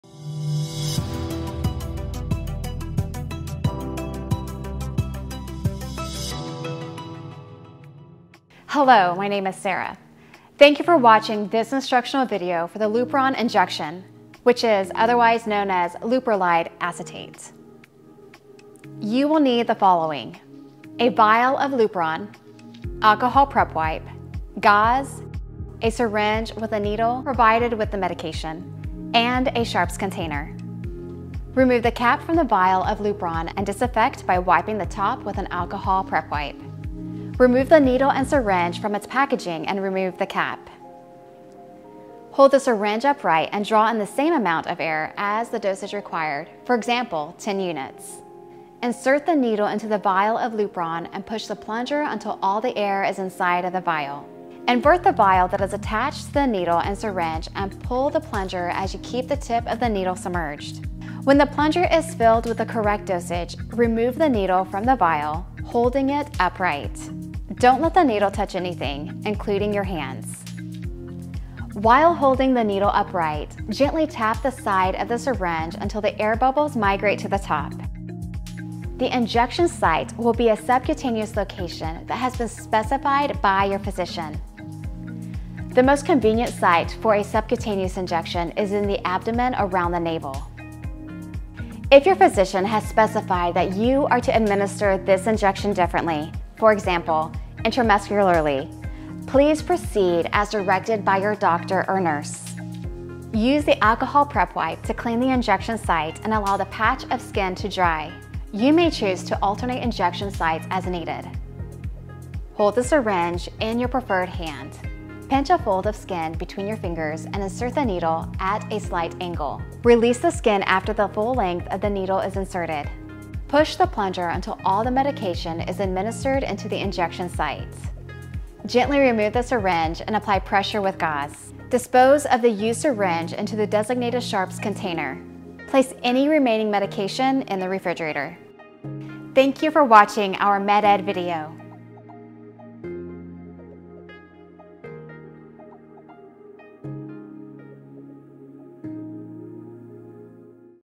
Lupron Injection Leuprolide Acetate 2 week kit Med-ed Instructional Video by ReUnite Rx
0:00 Introduction
0:19 Prepare materials
0:31 Draw up medication
1:11 Remove air bubbles
1:20 Prepare injection site
1:52 Inject medication
2:11 Dispose of needle
2:22 ReUnite website link

Lupron suppresses luteinizing hormone (LH) and prevents premature ovulation during fertility treatments.